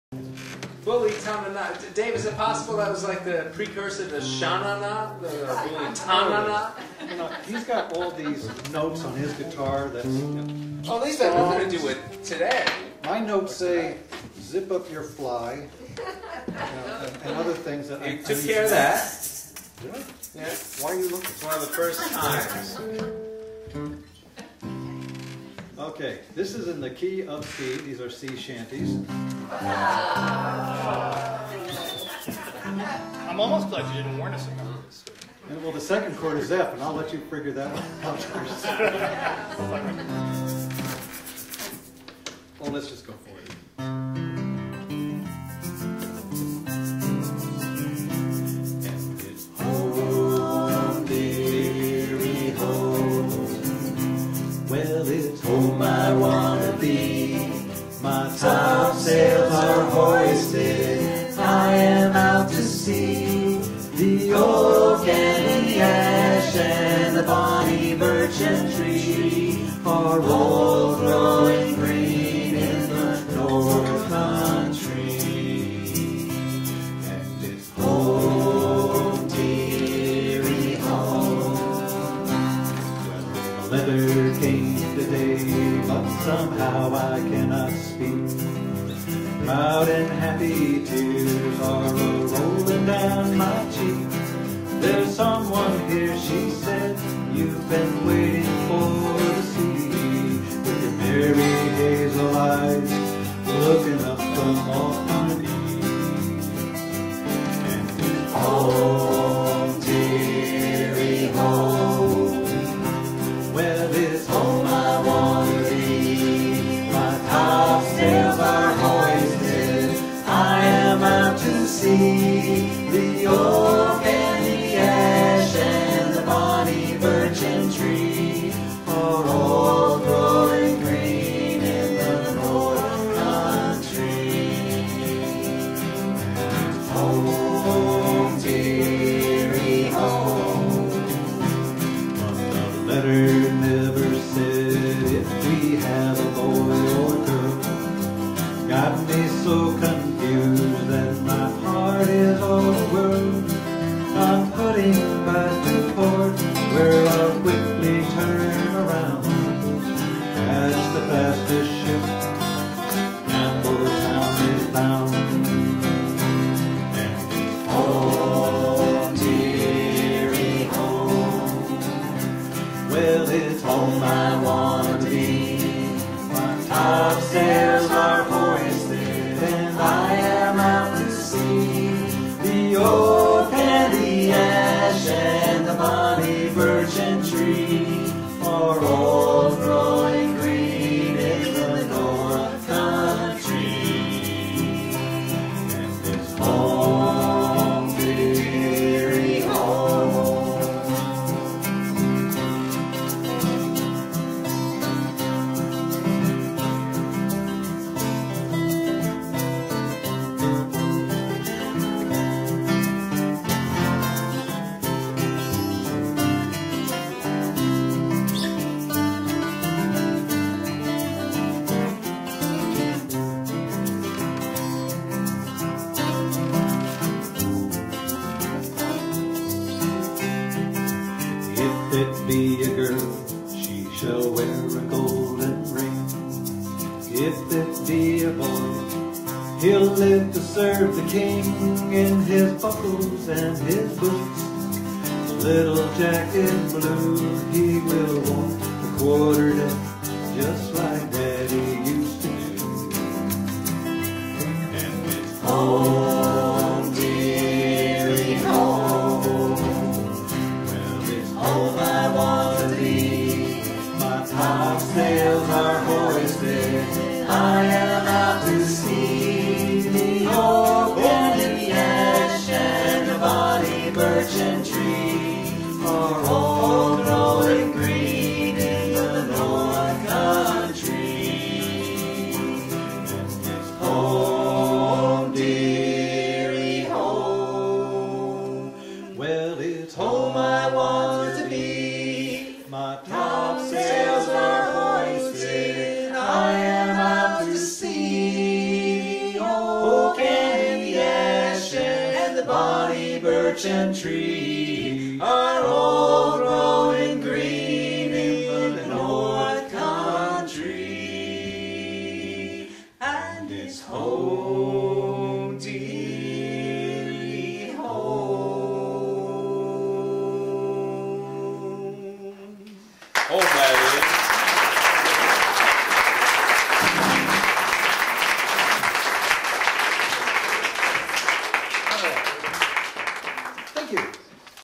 DOC HOWELLS AND FRIENDS 2
Members include Andrew Ferraro, Paula Howells, Jim Donnelly, Debra Martin, and David (Doc) Howells.